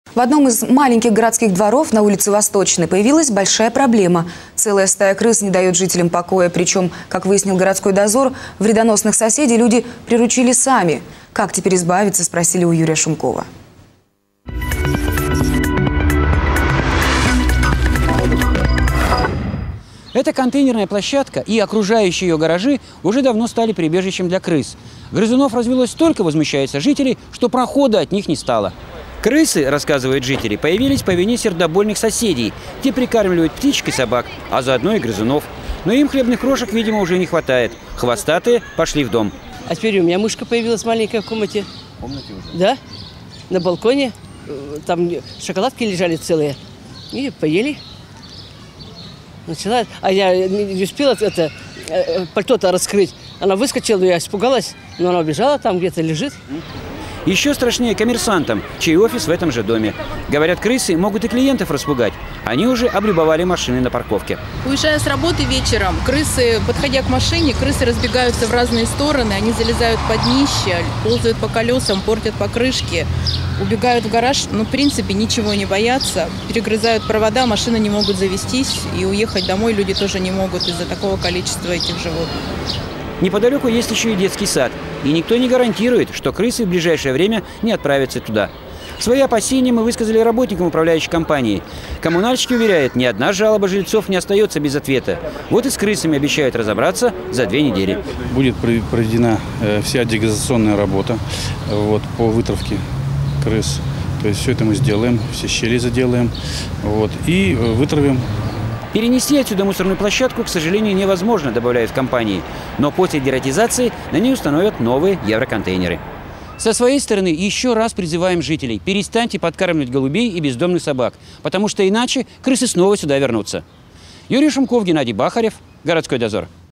Городской дозор. Крысы-соседи
Грызуны на улице Восточной не дают жителям покоя. Причем неприятных соседей люди приманили сами, подкармливая птичек и собак